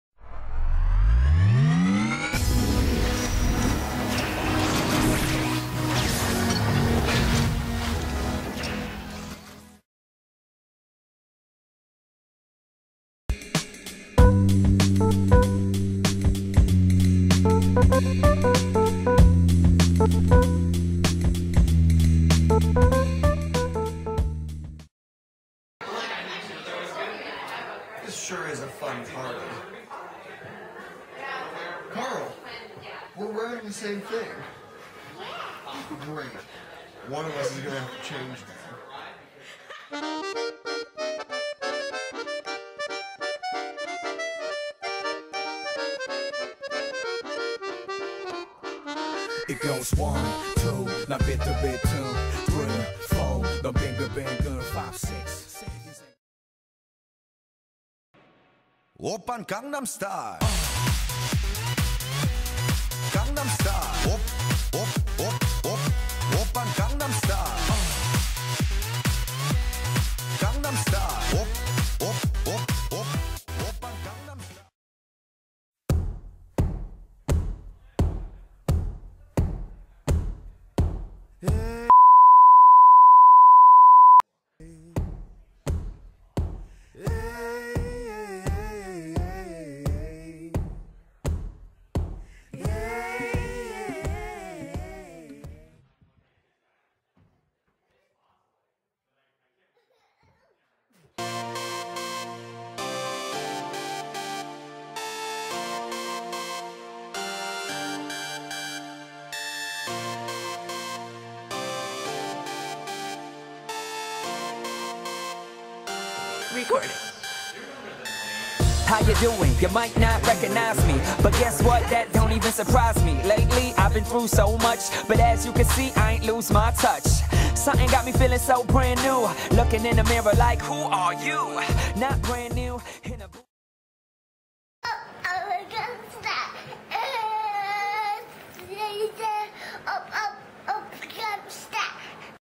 New Year's Party Fouls 2013
Was your Holiday season sprinkled with awkward get togethers-you know? The kind with ugly sweaters, or fancy clothes that don't quite fit right? That family dinner with tension so thick you could cut it with a knife like Marymoor on a foggy day? Or the New Year's bash with too much Martinelli's?
Well we certainly did.

HAPPY NEW YEAR FROM ALL OF US AT MSG ENTERTAINMENT!

Special thanks to our guest star: David Bander! (and don't worry. he didn't actually say anything worth bleeping. )

PLEASE, PLEASE, PLEASE SUPPORT THESE AMAZING ARTISTS!
(They're not only our jams, but heros, friends, inspirations, and icons)